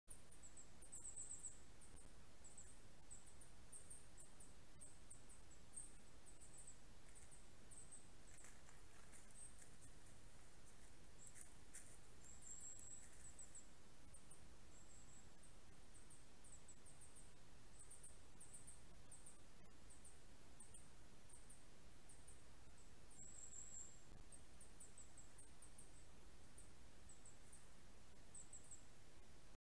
Suur konnakotkas Clanga clanga 2021 07 27 10 18
The chick exercises its wings and loses balance